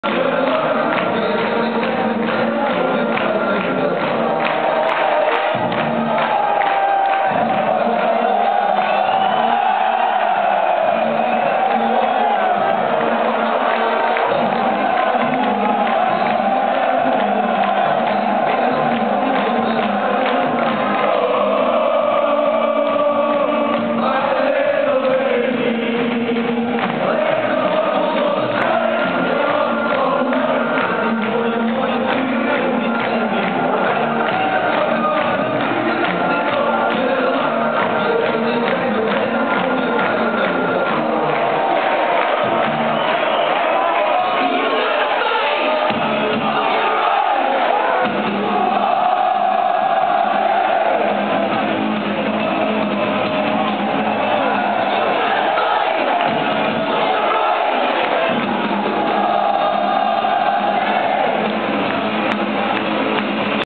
Willem II - FC Groningen Seizoen 08-09. Stand 3-0. Spelers bedanken publiek.
Spelers bedanken het publiek in de sneeuw na de gewonnen wedstrijd tegen Fc Groningen (3-0) seizoen 08-09